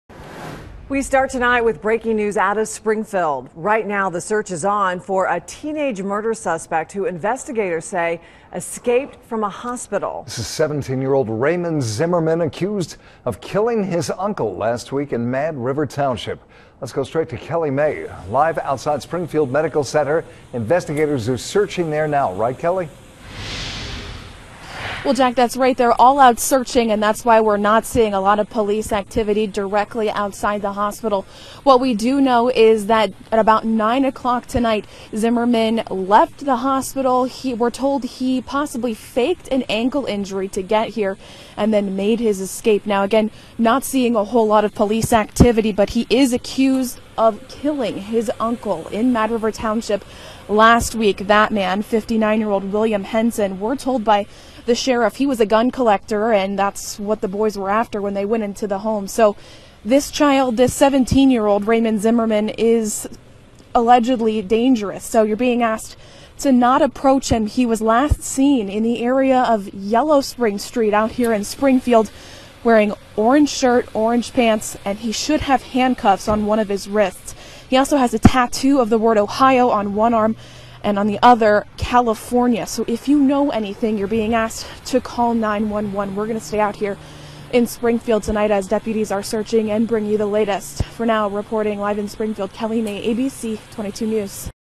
Teen Murder Suspect on the Run in Springfield
SPRINGFIELD -- Police are looking for a teen murder suspect who escaped Friday night from a Springfield hospital.

Raymond Zimmerman, 17, is accused of killing his uncle in Clark County last week. He and 18-year-old Robert Winbush were arrested in connection to the death.

Investigators say Zimmerman faked an ankle injury and was then able to escape around 9 p.m. Friday from Springfield Medical Center.

He is wearing an orange pull-over shirt and orange pants and has handcuffs on one wrist.